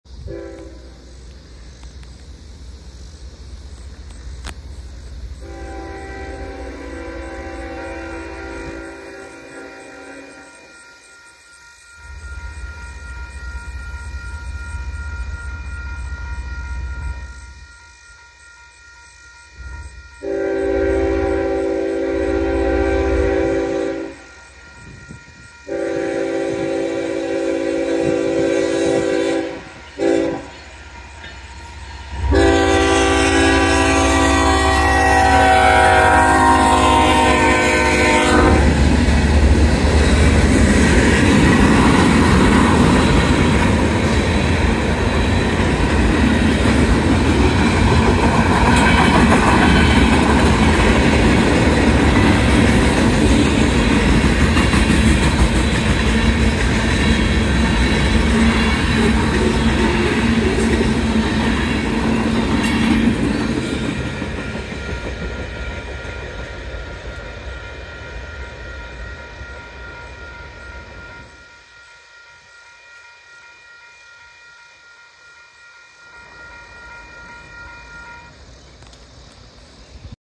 UP 7376 Short Manifest Freight Train East, 7th. St. Railroad Crossing, Waterloo NE
Filmed 8:29 PM Wednesday, August 30, 2023.

Union Pacific Railroad - Short Manifest Freight Train -

UP 7376 = GE ES44AC - Flags - Raised Letter K5HL